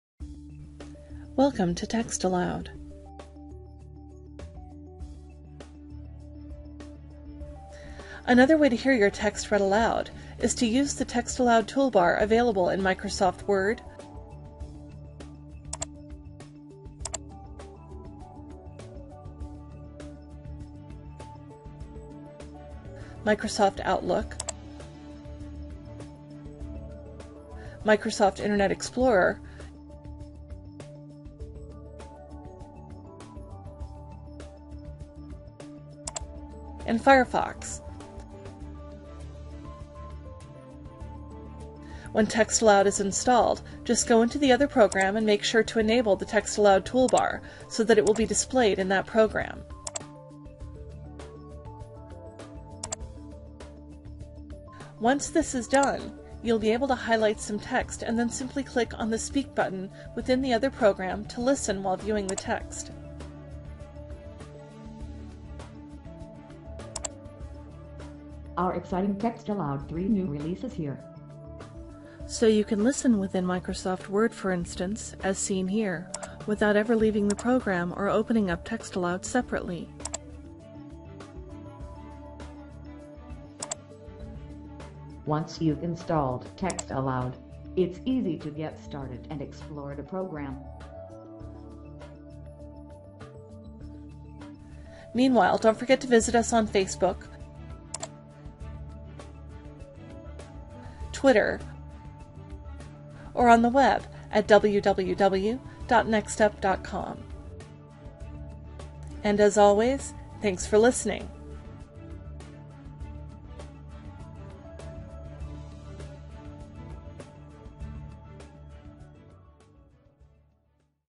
Configuring TextAloud 3 Toolbars in Other Programs
Once you've installed TextAloud, the program makes it easy to listen to your text at a moment's notice -- without opening the program itself, and from directly within many popular programs!  Thanks to its easily-enabled Toolbar add-ins, you can listen to your reading using TextAloud controls directly from such software programs as Microsoft Word, Microsoft Internet Explorer, Mozilla Firefox, Microsoft Outlook, and more!  And this simple video will show you how to do it in just moments.